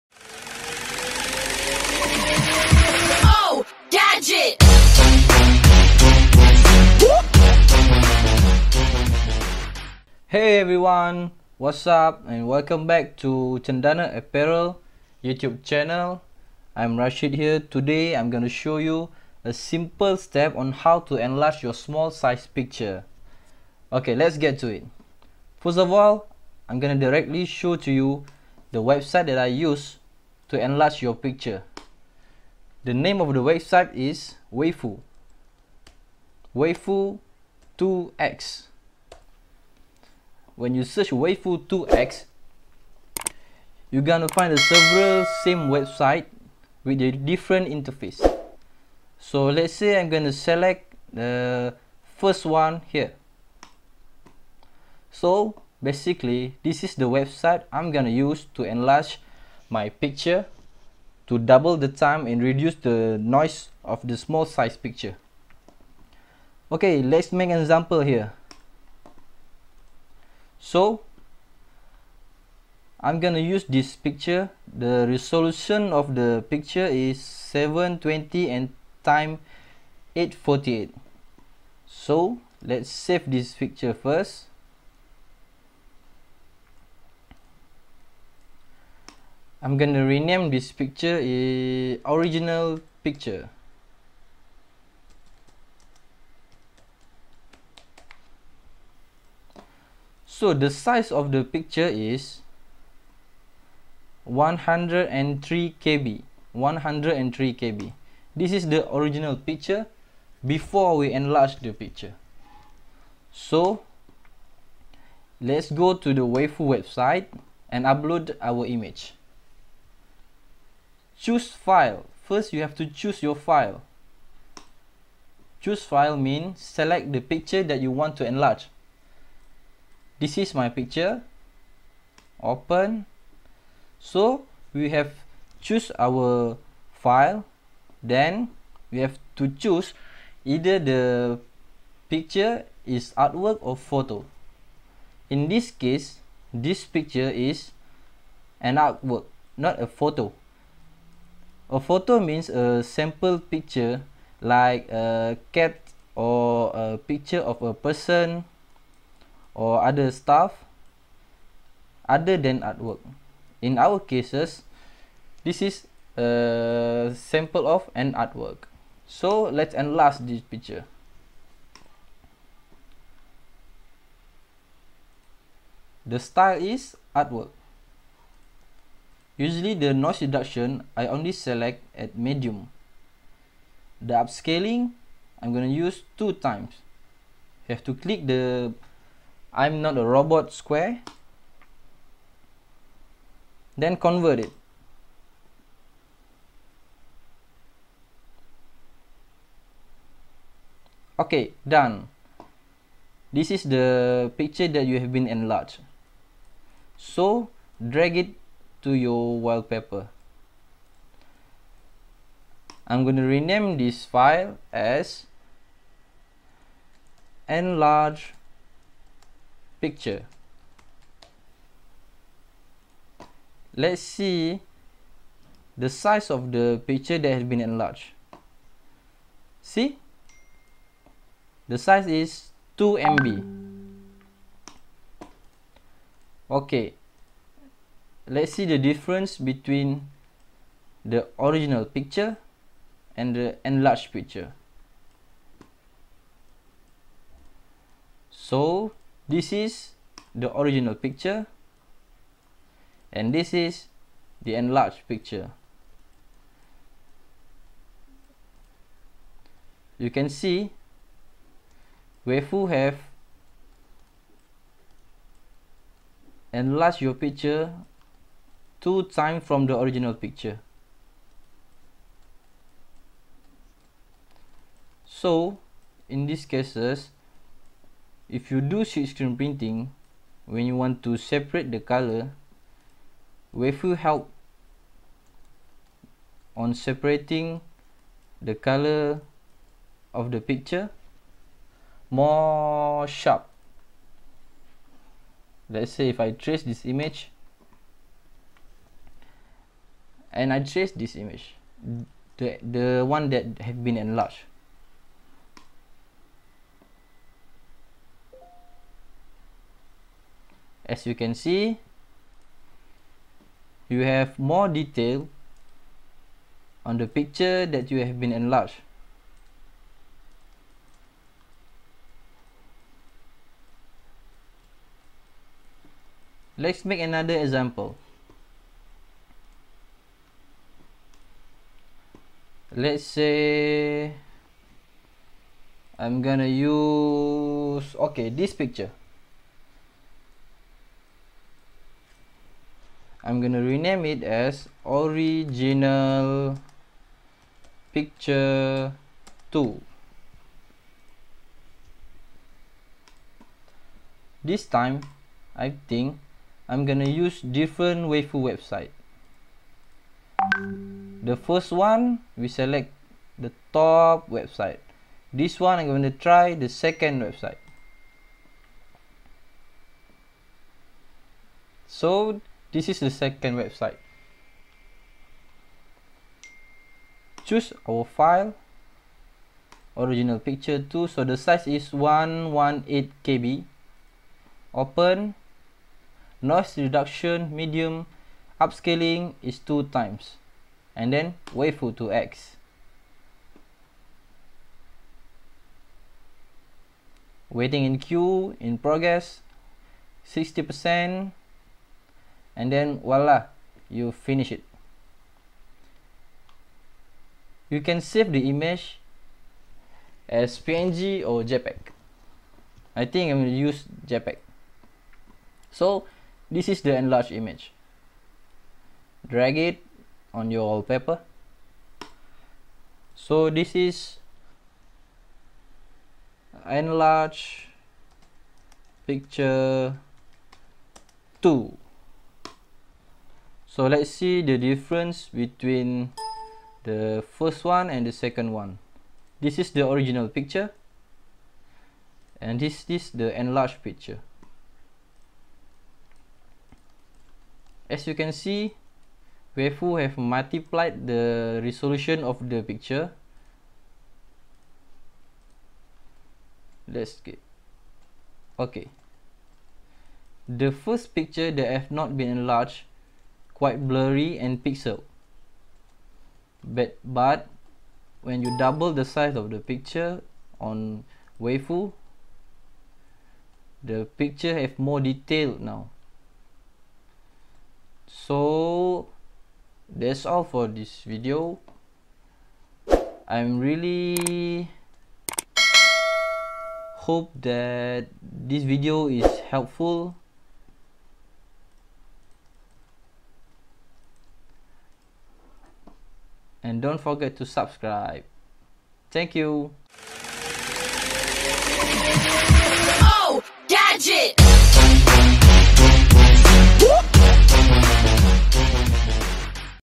HOW TO ENLARGE SMALL SIZE IMAGE. EASIEST WAY TO ENLARGE SMALL SIZE IMAGE OR PICTURE.
Hi guys.

In this video i 'll show you guys on how to enlarge your small size image.

If one of it is not working please try the other.

Hope this helps you guys!